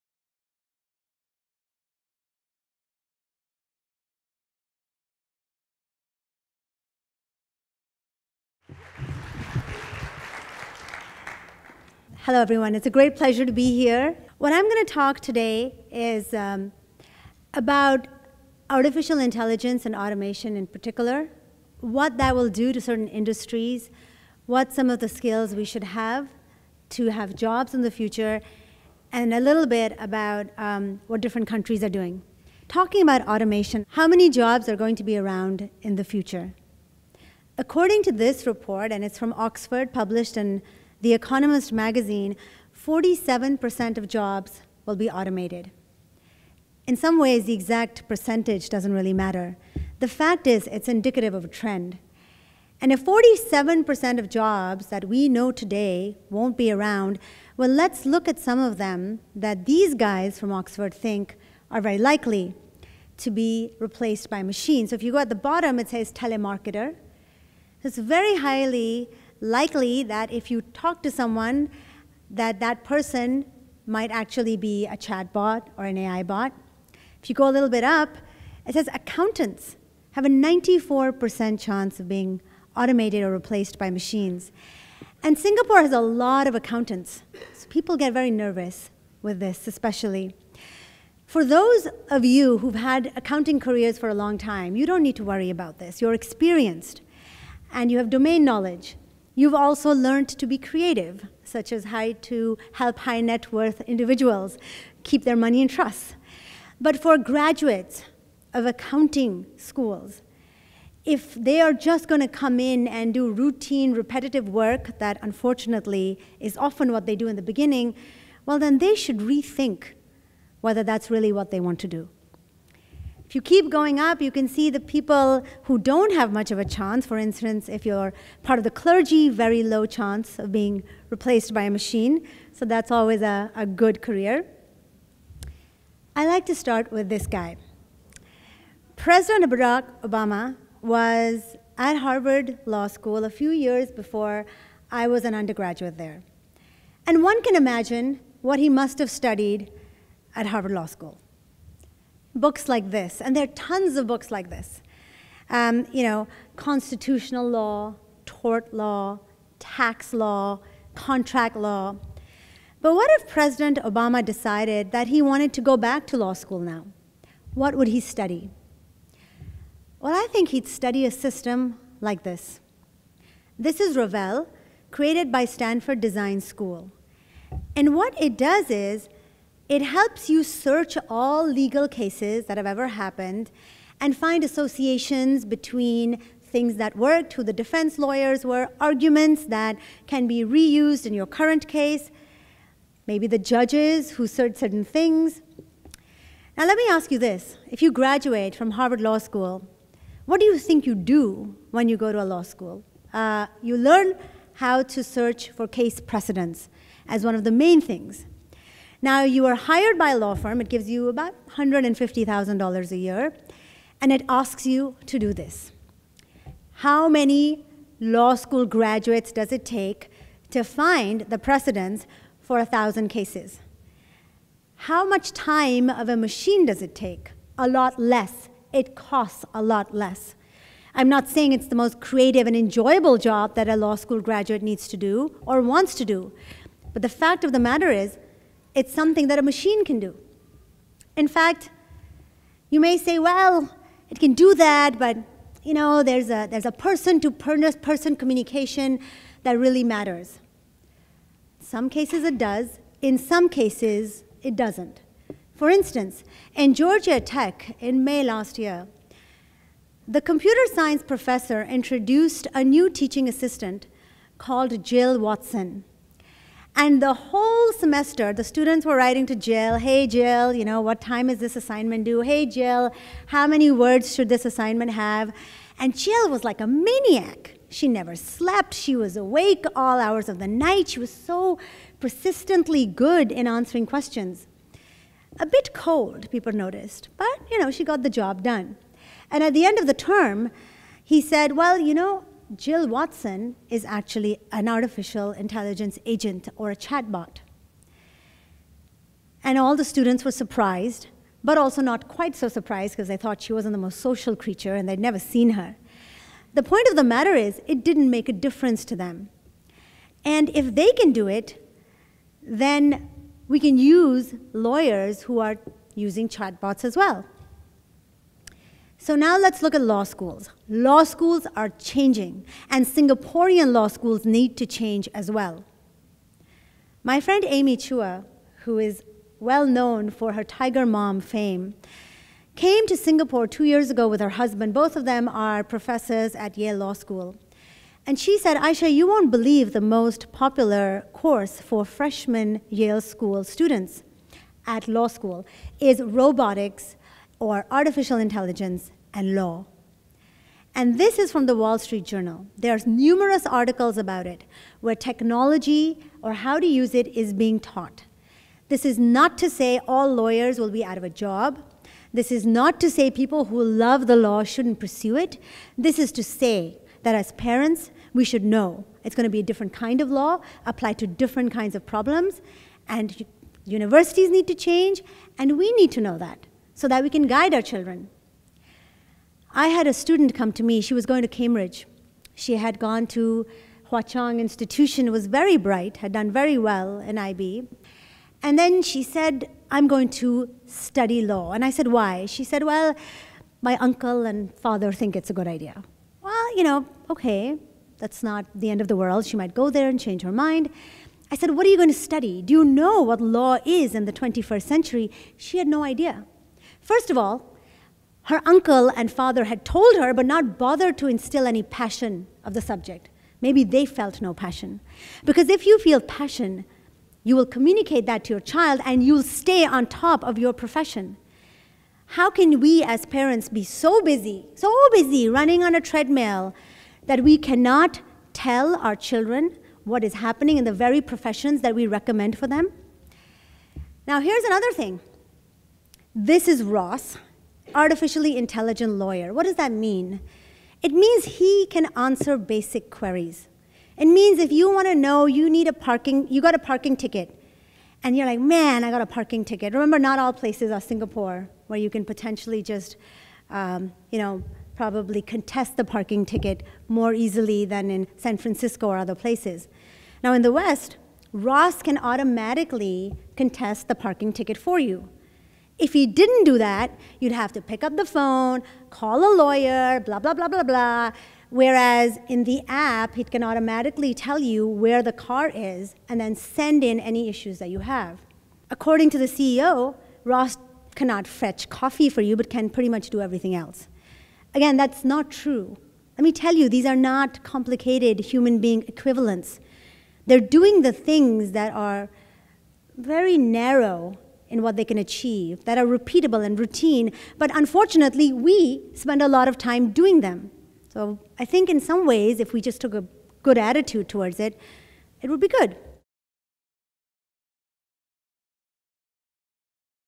Ayesha Khanna @ SmartGen 2017 (Part 1) - Main Conference
Skills of an age of automation